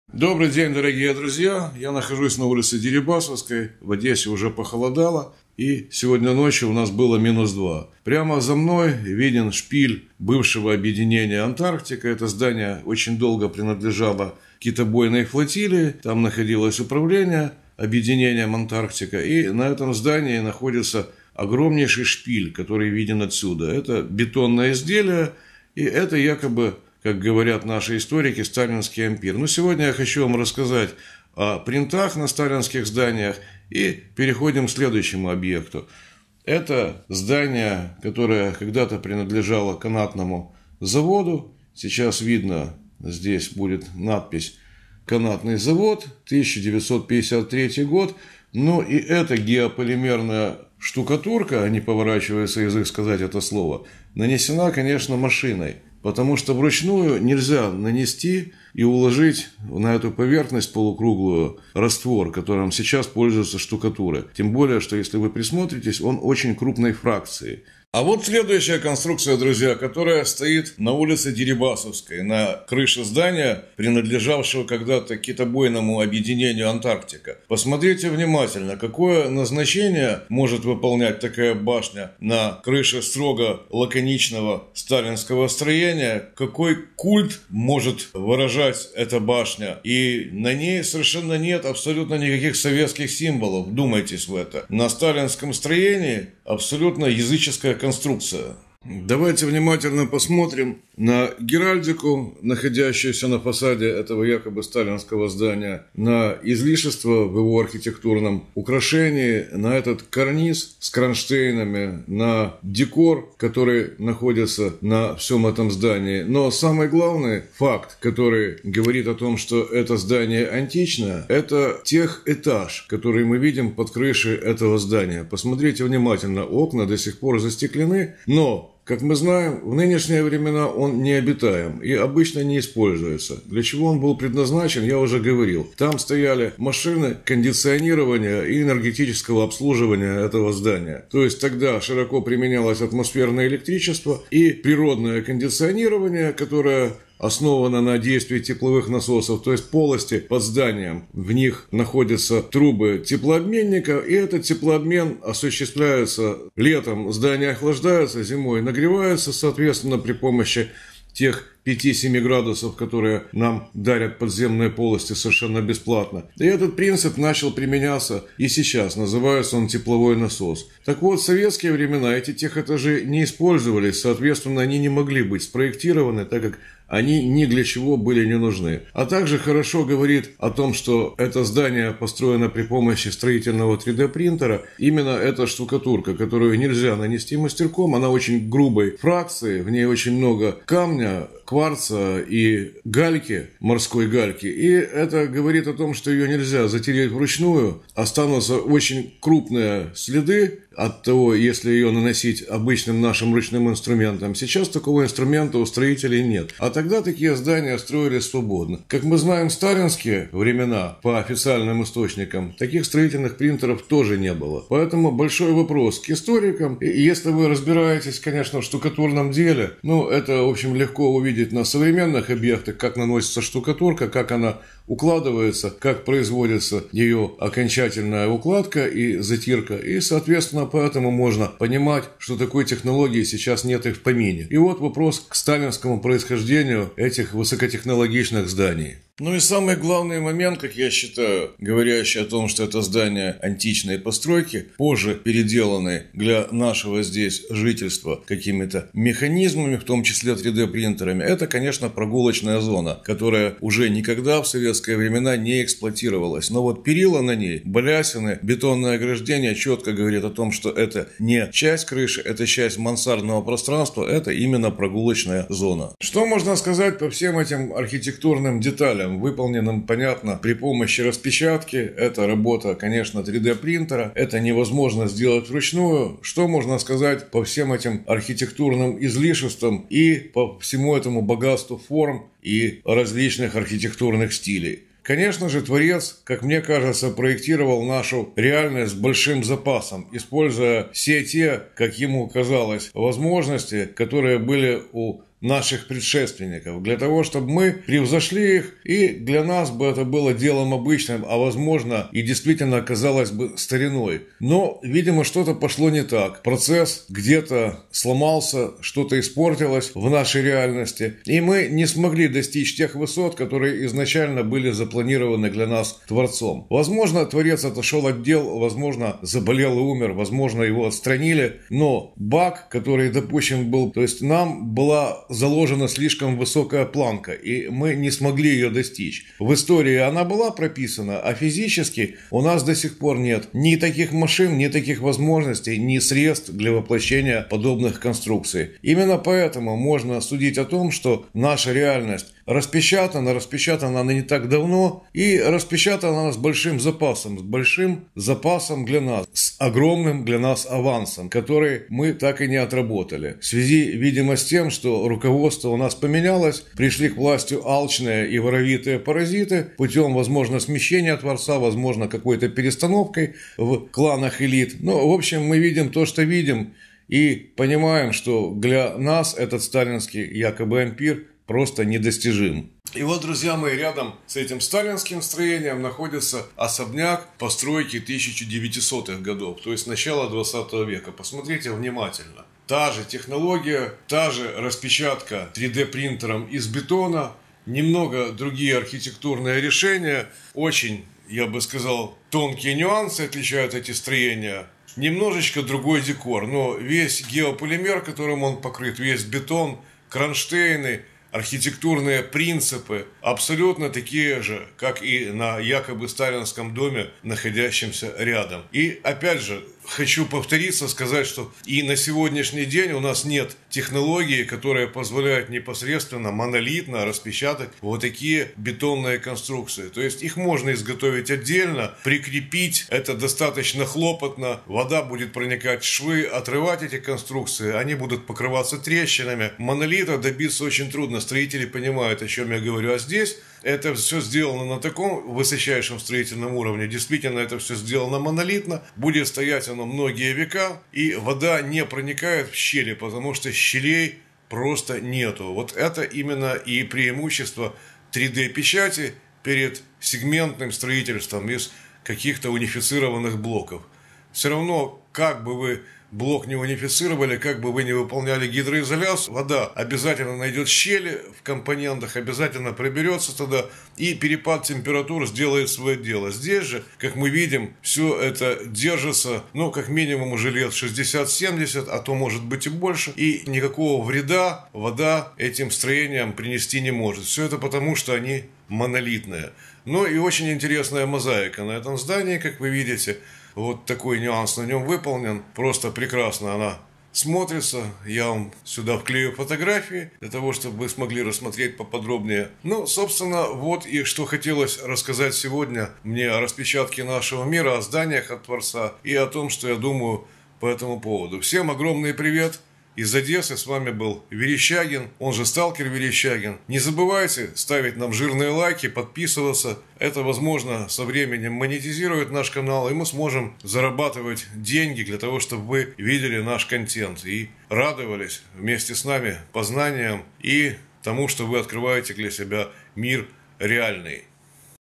Кто распечатал наш мир. Следы архитектора. Сталинские 3D принтеры
наша реальность следы распечатки геополимерные 3D принты. строительные технологии непостижимые и недоступные нам и сейчас. античный мир построенный заново архитектором в виде сталинских домов